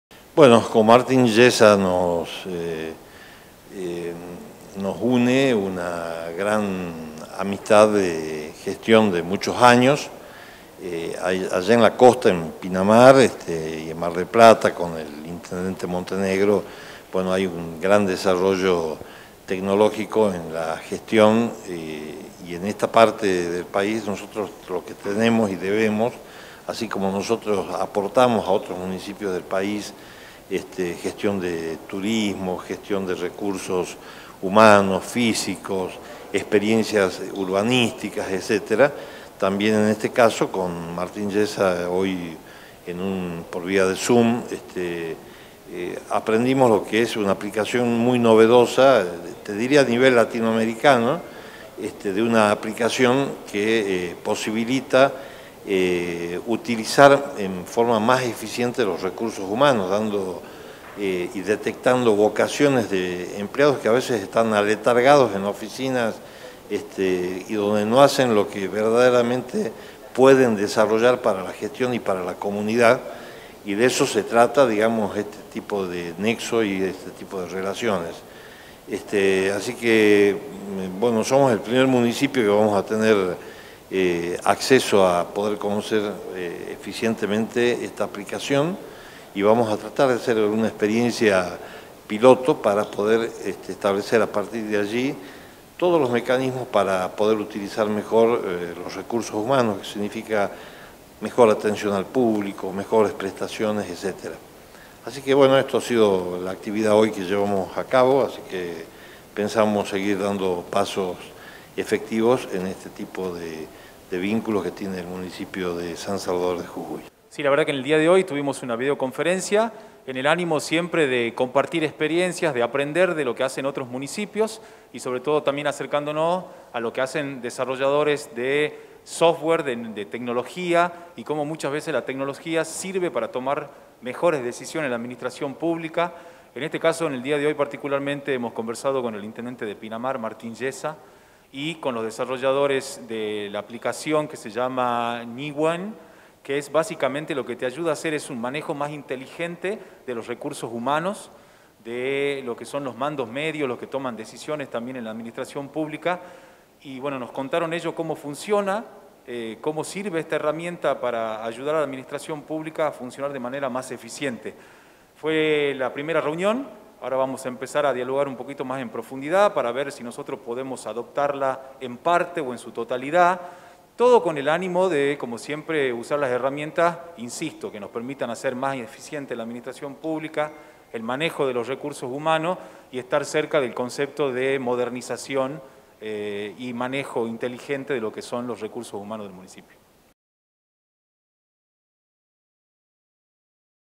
El Municipio Potenciará los Recursos Humanos
El Intendente, Raúl “Chuli” Jorge, junto a parte del gabinete municipal, realizaron una videoconferencia con el Intendente de Pinamar, Martín Yeza y desarrolladores de software, con el objeto de interiorizarse sobre la aplicación de un programa informático que posibilita la utilización eficiente de los recursos humanos en la administración pública.